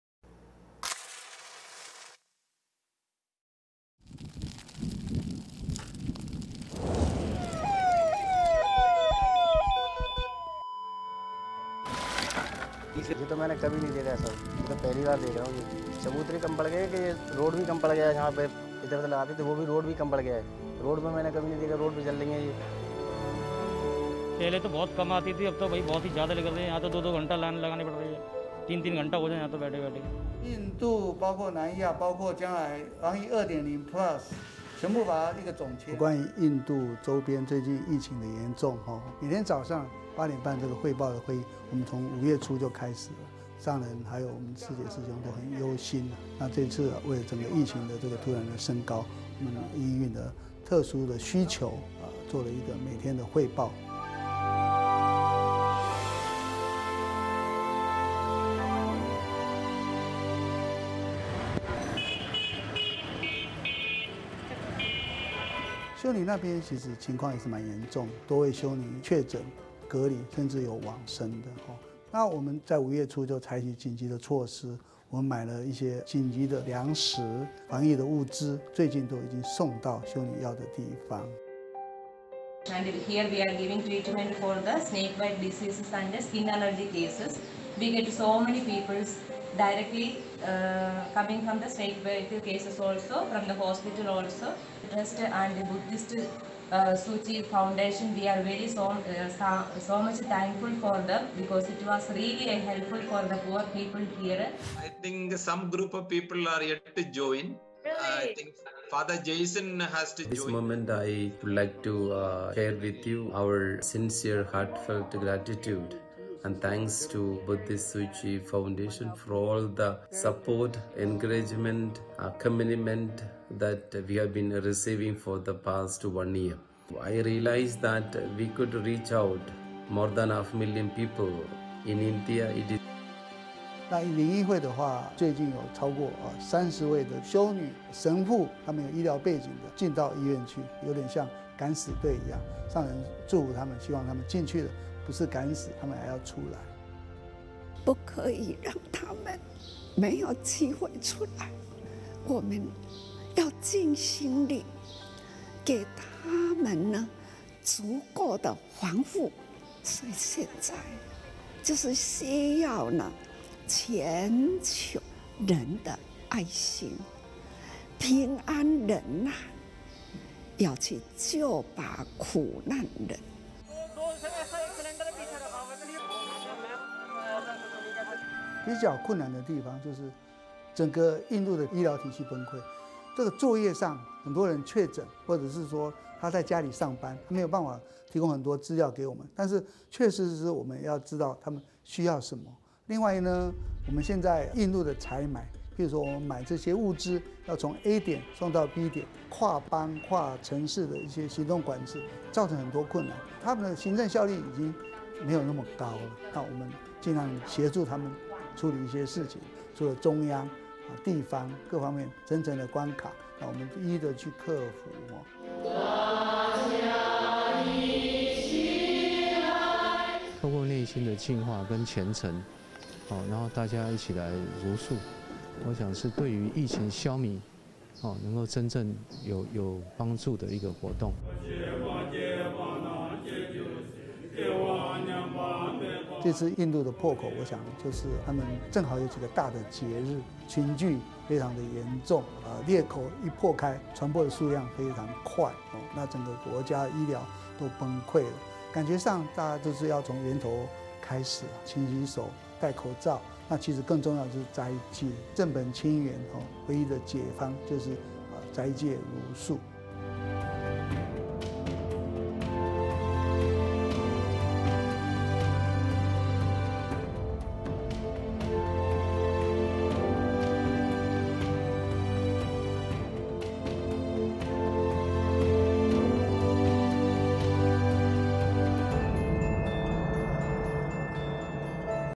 全球一命！慈濟與慈善組織支援印度
印度感染新冠肺炎，累計人數跨越2600萬大關，世界排名第二，天主教靈醫會與仁愛修女會，從去年開始，持續提供醫院防疫物資，同時也與當地慈善組織機構合作，陸續提供物資到偏鄉。而最近，印度鄰近幾個國家，疫情也升溫，同樣提出求援，慈濟全球共善，支援防疫。

印度截至2021年5月中，確診數超過2400萬，死亡數突破26萬人。

印度民眾 丁納許：「從沒看過像這樣火葬場爆滿，火化遺體的平台都不夠用，連馬路邊都堆滿，沒看過遺體像這樣堆到路上。」

救護車駕駛 歐姆：「我們載的都不是活人，而是新冠死者的遺體，至少還要等上兩小時才能火化。」

慈濟慈善基金會副執行長 熊士民：「有關於印度周邊，最近疫情的嚴重，每天早上8點半會報的會議，我們從5月初就開始了，上人還有師姊師兄都很憂心 ，這一次為整個疫情突然升高 ，我們因應特殊需求，做了一個每天的會報。」

2020年起，印度天主教仁愛修女會與靈醫會，與慈濟發放防疫物資。

慈濟慈善基金會副執行長 熊士民：「修女那邊情況也是滿嚴重的，多位修女確診、隔離，甚至有往生的，那我們在5月初就採取緊急措施，我們買了一些緊急糧食、防疫物資，最近都已經送到修女要的地方。」

截至2021年5月，慈濟全球COVID-19防疫行動，援助92國家地區。

印度修女：「我們照顧的病人都是窮人，特別是農民，這裡有太多的窮人，來了好幾次義診，印度靈醫會和佛教慈濟，我們非常感恩有你們。」

2021年5月4日，慈濟與印度靈醫會，進行援助視訊討論。

印度靈醫會 巴比爾神父：「值此時刻，我想與您分享，我們最誠摯衷心的感恩，感恩佛教慈濟基金會過去一年來給予我們的支持、鼓勵和陪伴。這讓我發現，我們在印度可以援助超過百萬人。」

慈濟慈善基金會副執行長 熊士民：「那靈醫會的話，他們最近有超過30位修女、神父，他們有醫療背景的進到醫院去 ，有點像敢死隊一樣，上人祝福他們，希望他們進去了，不是敢死，他們還要出來。」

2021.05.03 證嚴上人開示：「不可以讓他們沒有機會出來，我們要盡心力，給他們足夠的防護，所以現在就是需要全球人的愛心，平安人要去救拔苦難人。」

慈濟慈善基金會副執行長 熊士民：「比較困難的地方是，整個印度醫療體系崩潰，作業上很多人確診，或是說他在家裡上班，沒辦法提供很多資料給我們，但確實是要知道他們需要什麼。另外呢，我們現在印度的採買，買這些物資，要從A點送到B點，跨邦、跨城市，行動管制造成很多困難，他們的行政效率已經沒那麼高了，我們盡量協助他們處理一些事情，除了中央、地方，各方面層層關卡，我們一一去克服。」

2021年5月6日，靜思精舍為全球祈福，25個國家地區同步連線。

慈濟慈善基金會執行長 顏博文：「透過內心淨化跟虔誠，大家一起來茹素，我想是對於疫情消弭，真正有幫助的一個活動。」

2021年5月12日，捐防疫物資給南印度色拉傑寺院，法師誦經回向祈福。2021年5月18日，製氧機等防疫物資，送達馬囊區域醫院，及五個鎮的醫療院所。

慈濟慈善基金會副執行長 熊士民：「這一次印度的破口，我想就是他們正好幾個大節日，群聚非常嚴重，裂口一破開，傳播數量非常快，整個國家醫療崩潰了，感覺上大家從源頭開始，勤洗手，戴口罩，其實更重要就是齋戒，正本清源，唯一解方就是齋戒茹素。

慈濟全球共善，2020年印度疫情援助，16省分、13機構、19萬戶次、94萬人次。截至2021年5月18日，援助印度及周邊等7國，印度、尼泊爾、斯里蘭卡、不丹、孟加拉、寮國、柬埔寨，約61機構；援助內容預計：建製氧氣槽、3700台製氧機捐贈、2000個空鋼瓶等物資、260000戶次家庭糧食紓困。